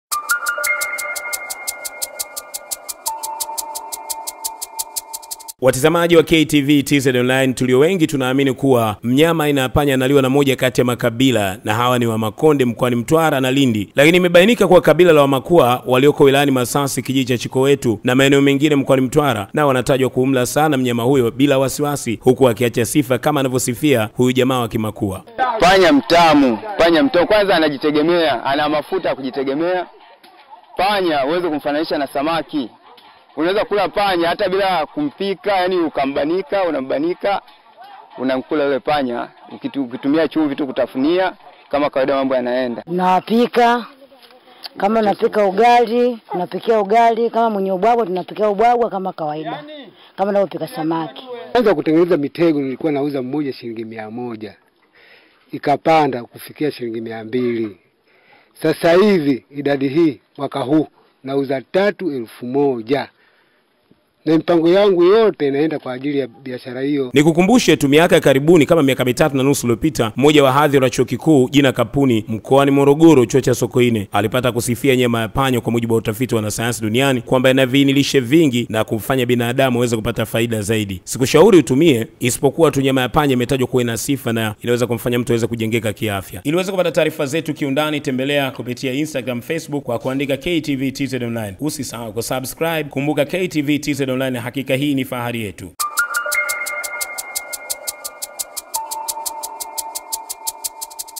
NYAMA YA PANYA YAWA DILI TANZANIA
Jamii ya wamakauwa  mkoani Mtwara wasifia nyama ya Panya kwa utamu na wameamua kutumia nyama hiyo baada yakukosa nyama  ya ngombe